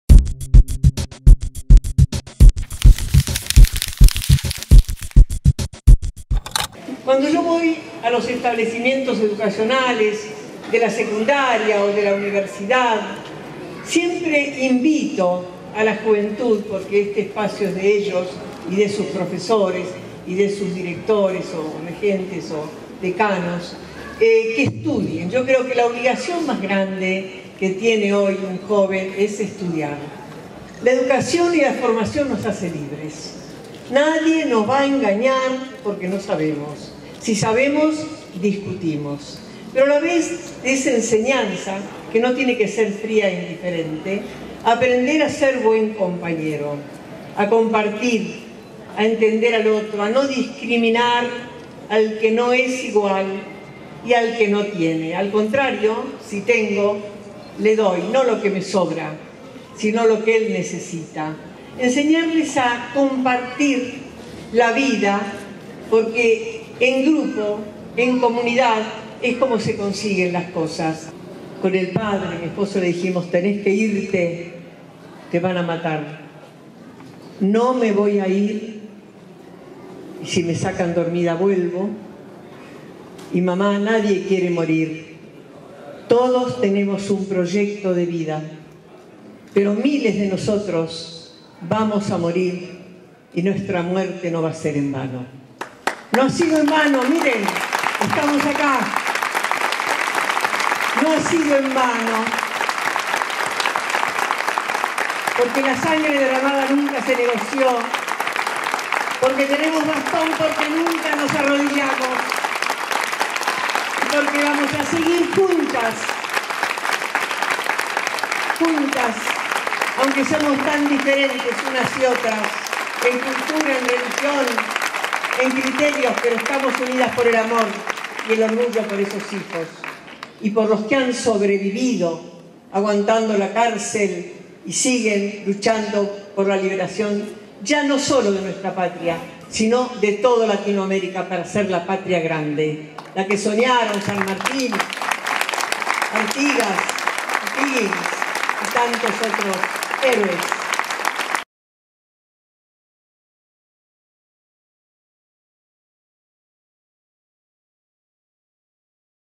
Carlotto: "tenemos bastón porque nunca nos arrodillamos"
La titular de Abuelas de Plaza de Mayo visitó Salta y habló sobre el balotaje. “No apoyamos partidos, sino gestiones”, expresó.